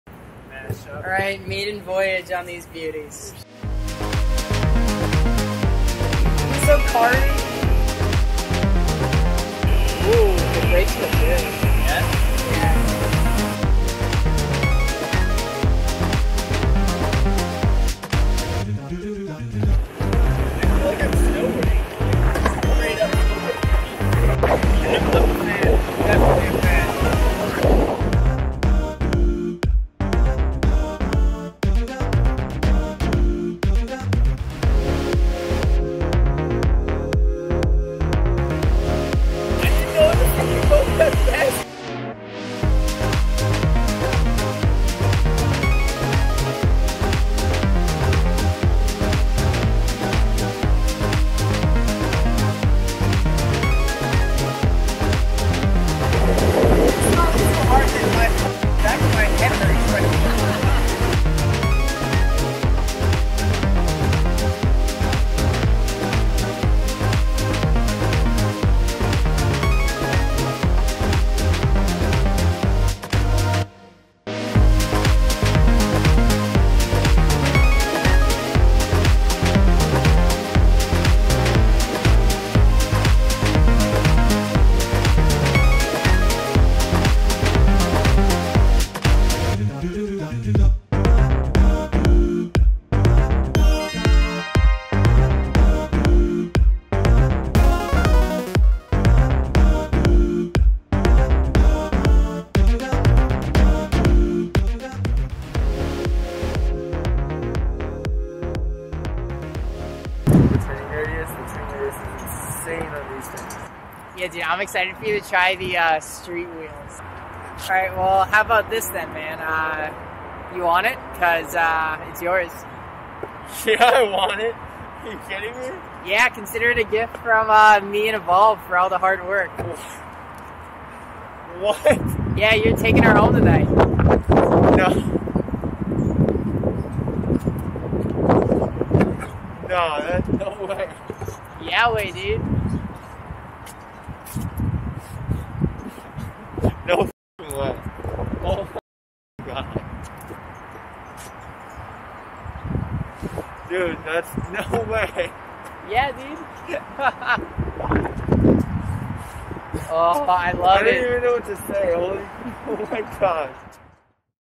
First ride on the Evolve Stoke and Carbon GTR! Then I gift the Carbon GTR to my friend!
We had a blast trying out the Evolve Stoke and Carbon GTR! As a surprise at the end of the video I gift the Carbon GTR to my good friend and cinematographer Suicide.Shots!